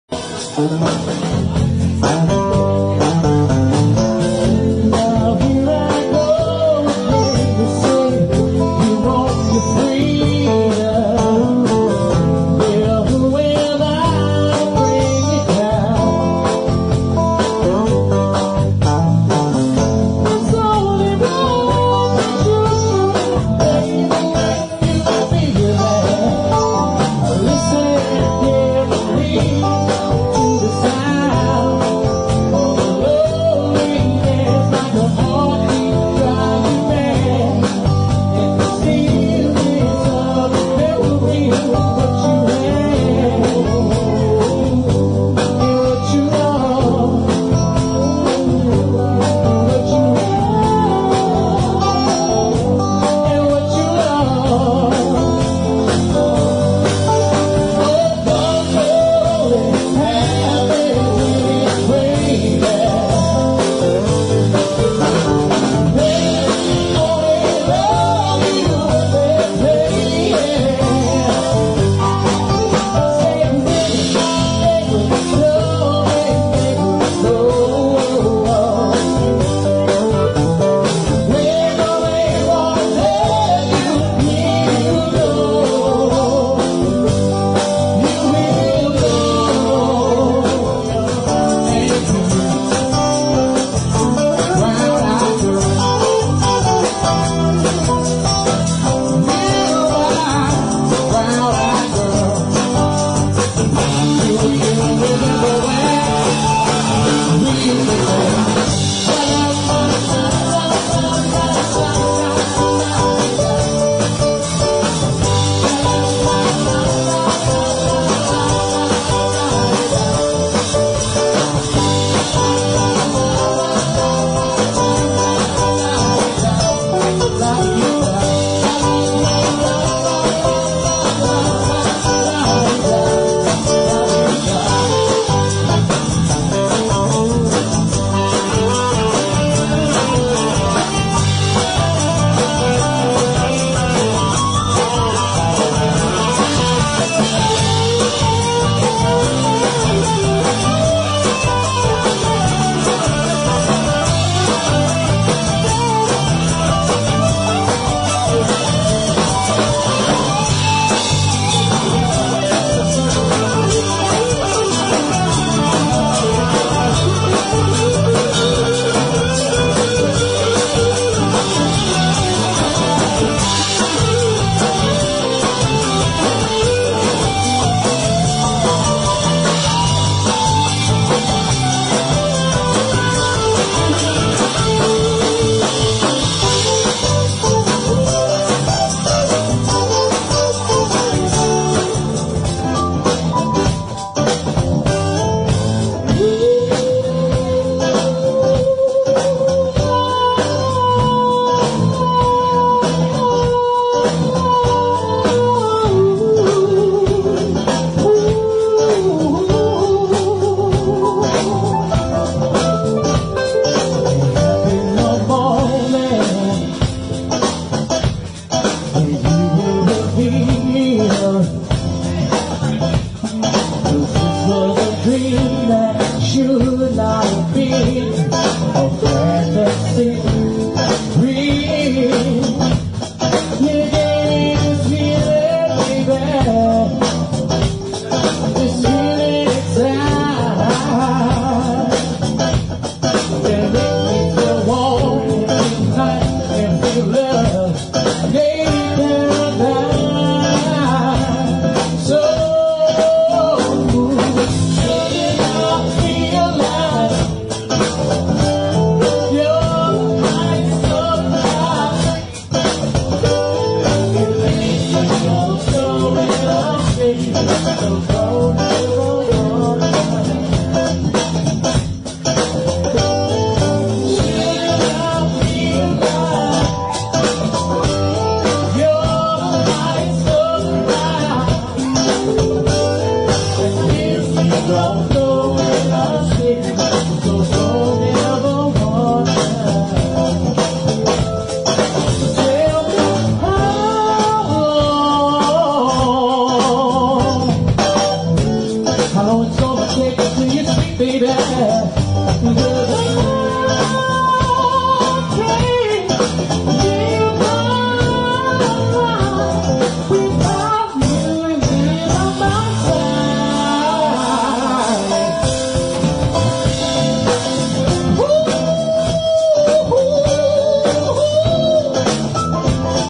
Electric Nation Band Playing At Sammy's Original in Lake Forest...
Electric Nation Band Playing Some Fleetwood Mac. Van Morrison & Maxwell Covers...

I Do Not Claim Ownership Or Copyright To Any Of The Music Played Here.